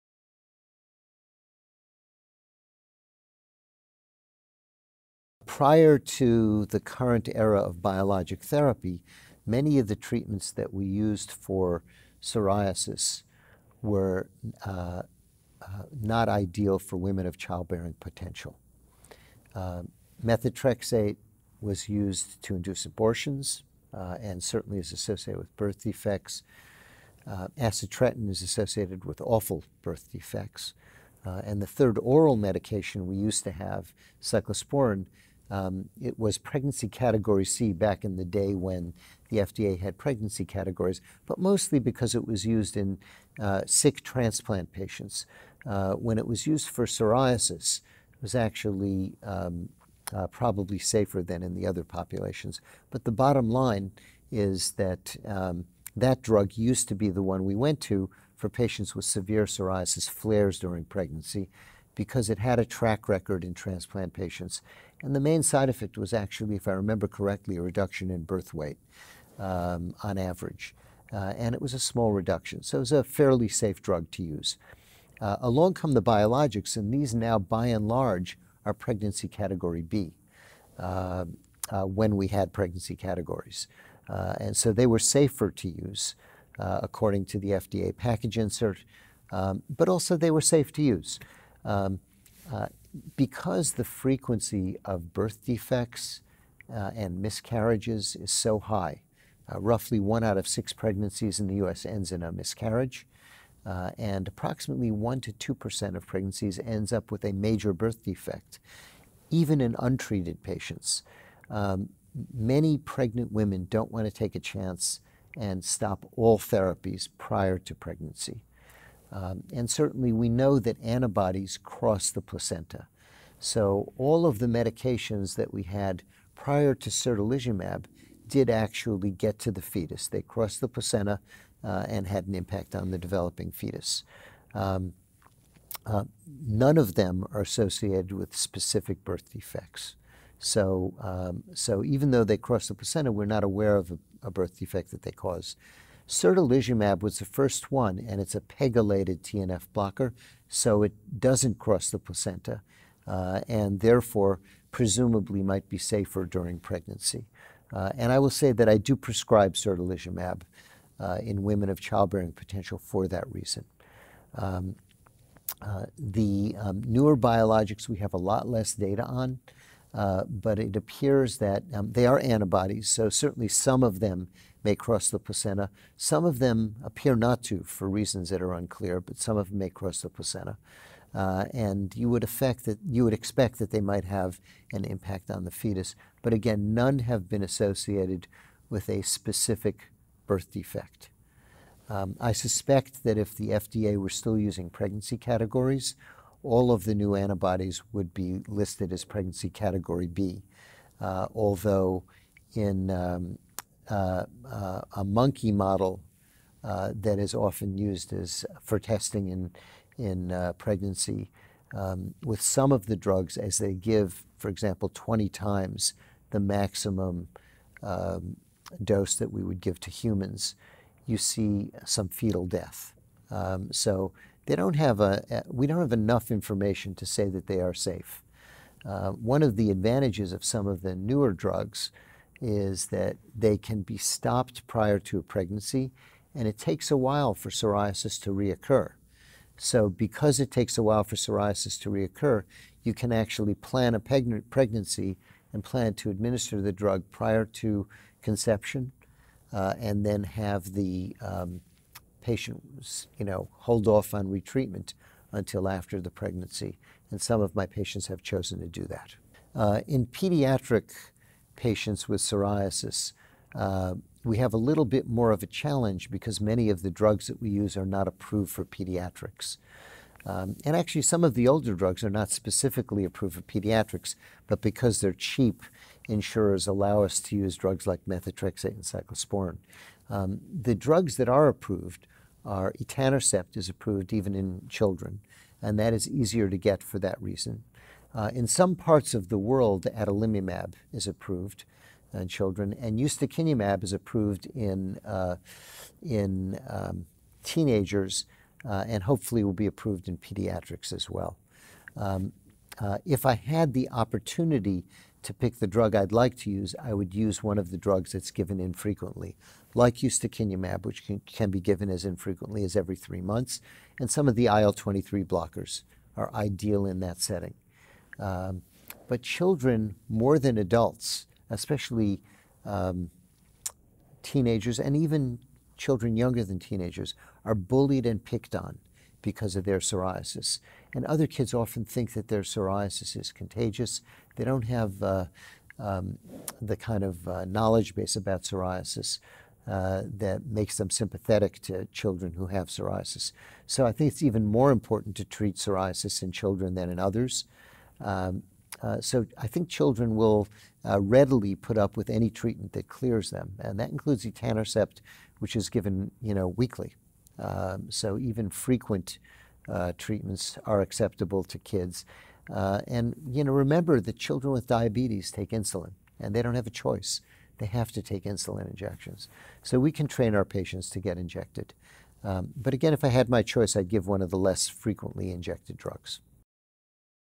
Treatment of Plaque Psoriasis in Specific Patient Populations
Treatment options for plaque psoriasis in women of childbearing age and the pediatric patient population.